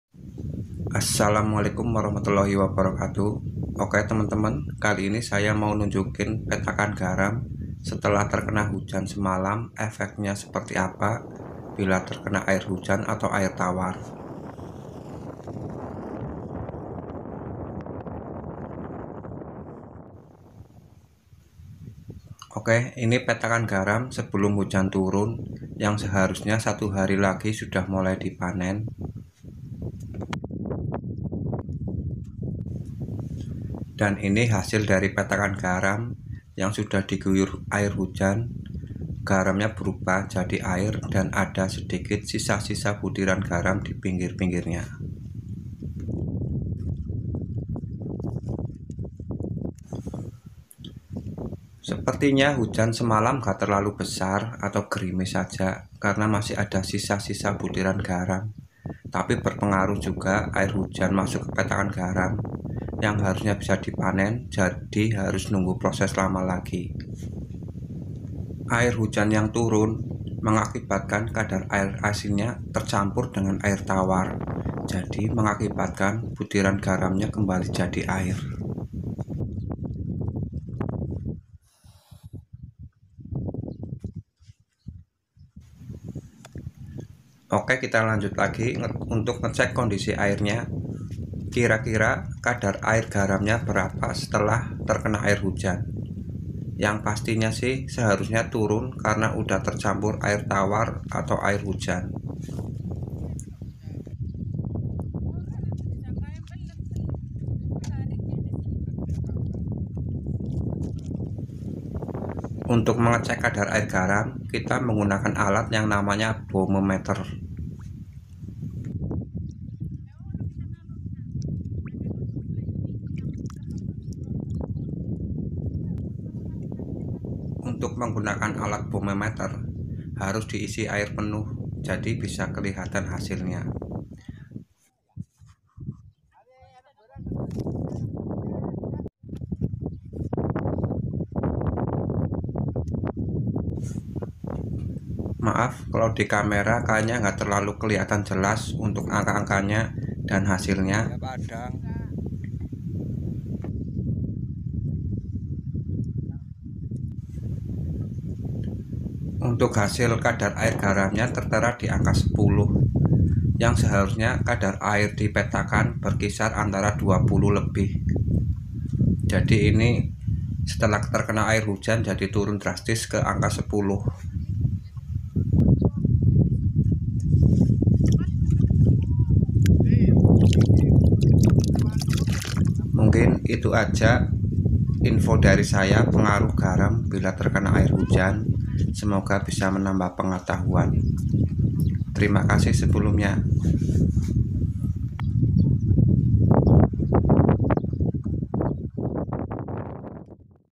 Pengaruh air hujan pada pembuatan garam
pengaruh air hujan pada proses pembuatan garam, air hujan mungkin berkah bagi semua orang, tetapi tidak pada petani garam, ketika musim panen garam yang diharapkan petani adalah panas matahari yang terus bersinar untuk proses penguapan garam, dan menghasilkan butiran butiran garam yang siap untuk dipanen dan dijual, tetapi ketika hujan turun proses pembuatan garam akan terhenti lama yang diakibatkan air hujan atau air tawar akan bercampur ke petakan air asin yang membuat garam menjadi air lagi, beda ketika disiram dengan air asin lagi. kadar air garam akan turun karena tercampur air tawar dan awan mendung berpengaruh juga pada proses penguapan, karena panas dari mataharinya berkurang.ketika air tawar bercampur dengan air asin untuk pembuatan garamnya membutuhkan waktu yang lama lagi untuk kembali ke kadar air asin yang ditentukan, karena itu mungkin air hujan berkah untuk semua orang tetapi menangis untuk para petani garam, tetapi efeknya juga harga garam jadi naik karena produksi garamnya terhenti stock garam tidak mencukupi.